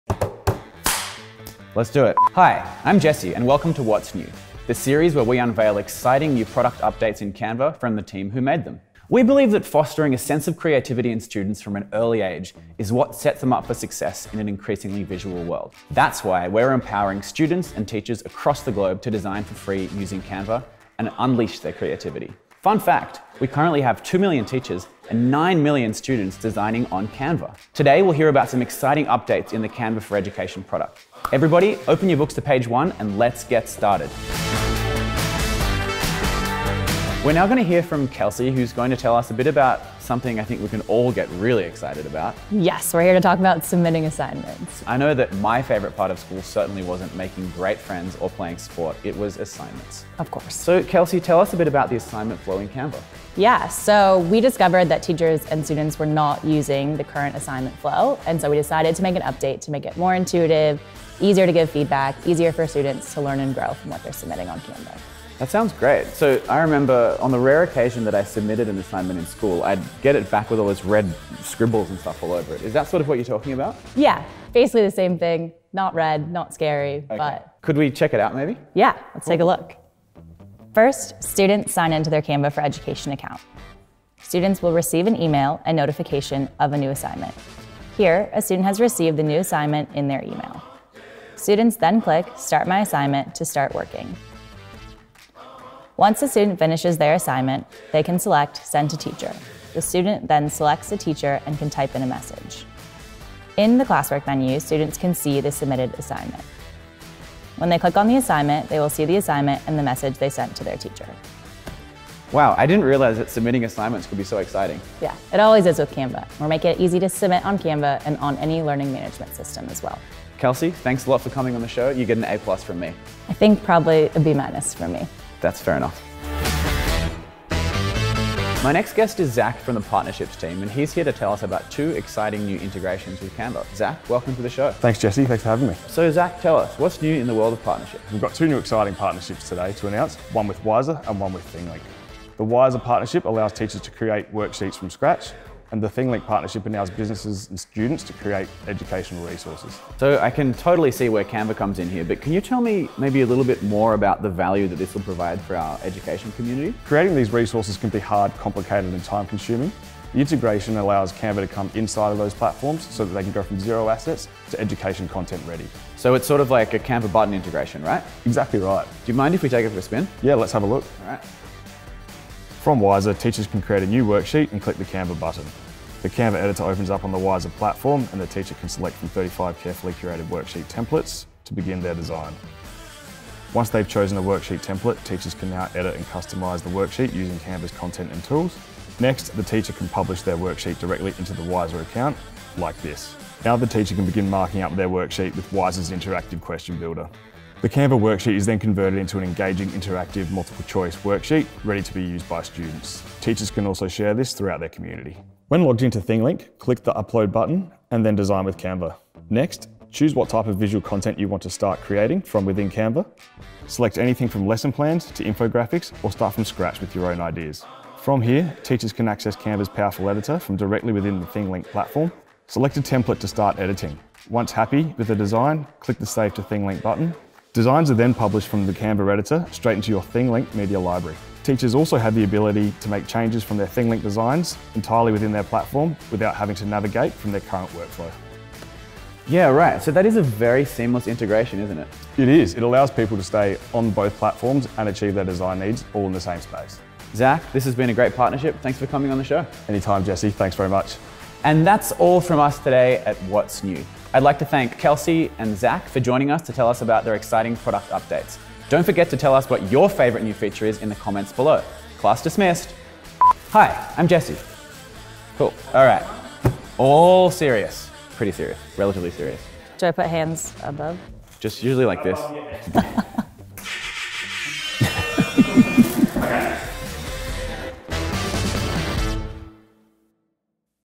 May 2021 | What’s New? | Canva
Teachers, listen up! Here’s your chance to learn about the new education features in Canva. Joining us for a chat this month are Ms. Moore and Mr. Thorneycroft. So, What’s New? 👇

📝 Submitting assignments the Canva way
🤝 Thinglink and Wizer partnerships for smooth lesson planning